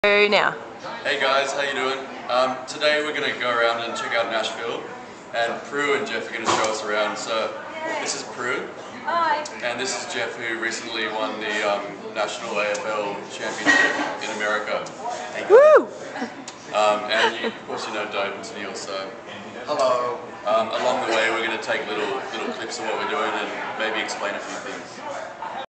Tour of Nashville
Luke & Tenielle prepare to tour Nashville, accompanied by Dobe Newton, Jeff Persson, President of the Nashville Kangaroos AFL team and Prue.